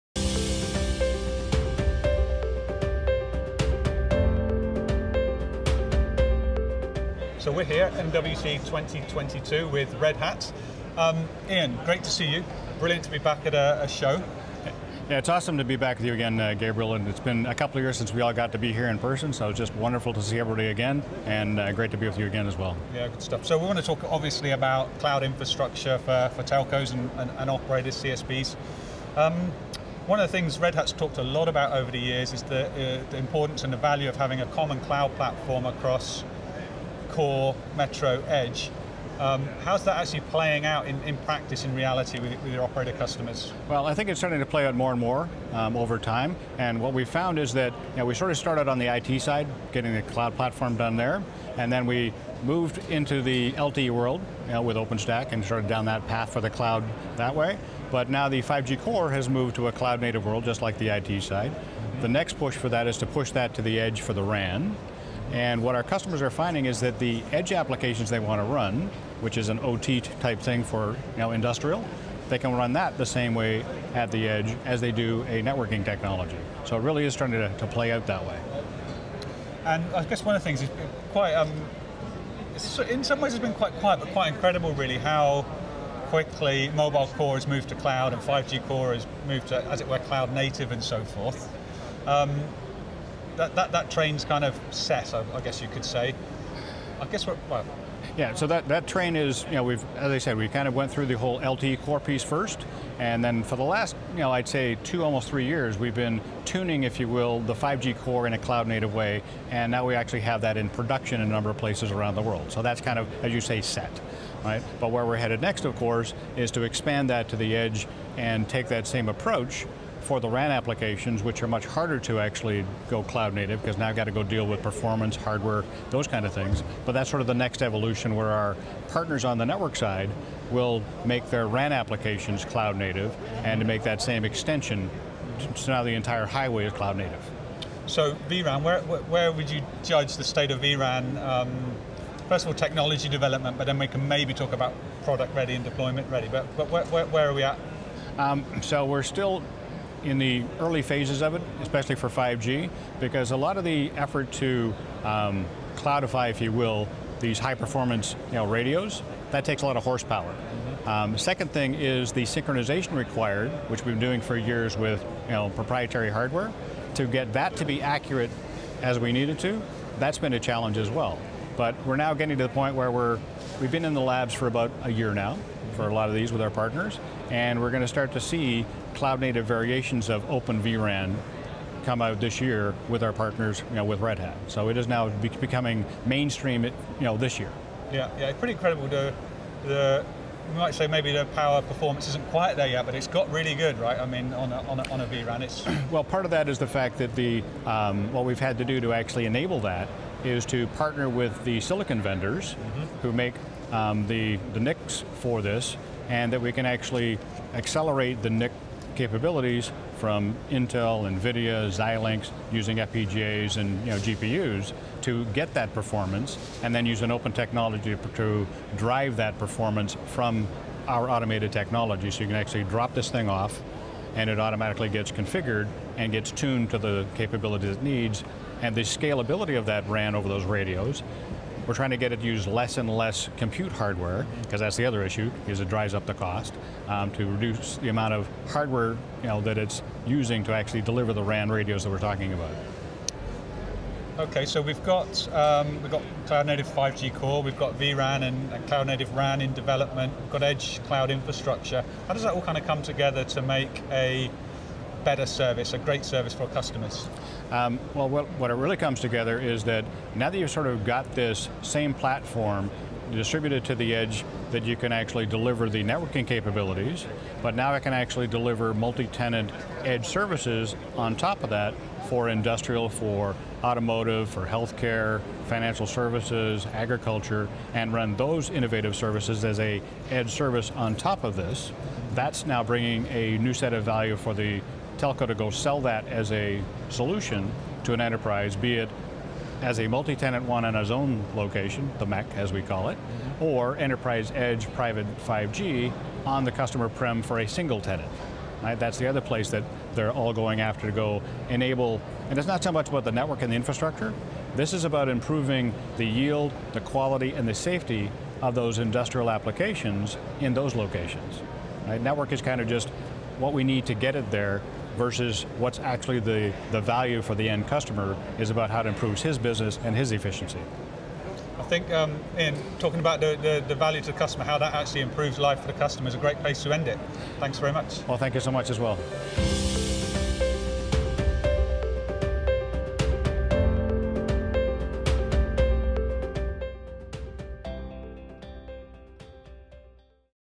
Building the open cloud native highway
Cloud native mainstream for 5G core is now extending to RAN at the edge.